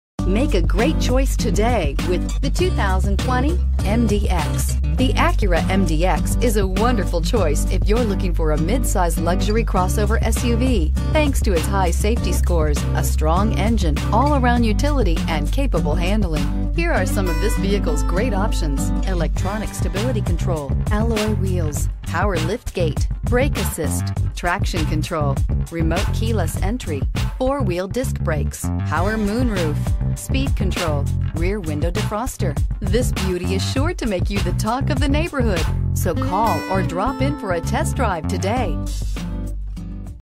2020 Acura MDX 3.5L 5133
2020 Acura MDX 3.5L

Sunnyside Acura
482 Amherst st

Modern Steel Metallic 2020 Acura MDX SH-AWD, 3.5L V6, 9-Speed Automatic.brbrRecent Arrival!